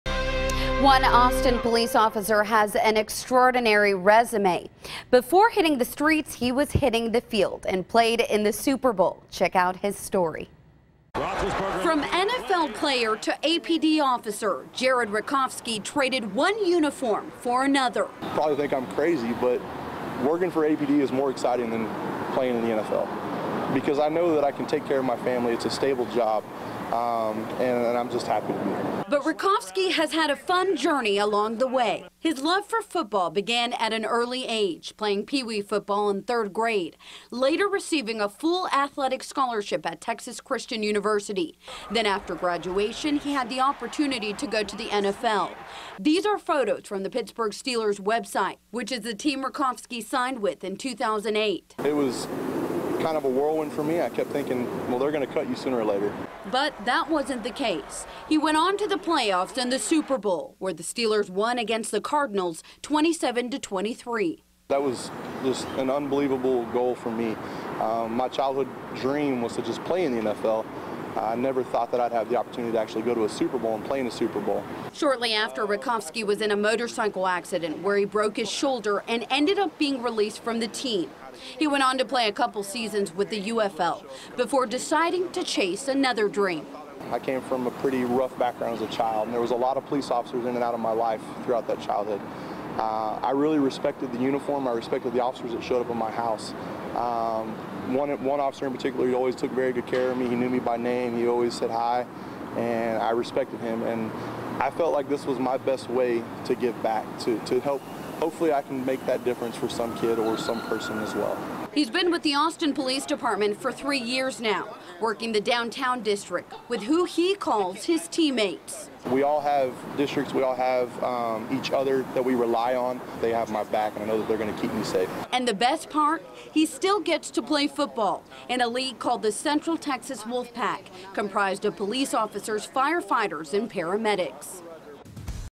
From the NFL to the Austin Police Department | 3/2017
One Austin police officer has an incredible resume. Before hitting the streets, he was hitting the field and played in the Super Bowl. FOX 7's Ashley Paredez has the story.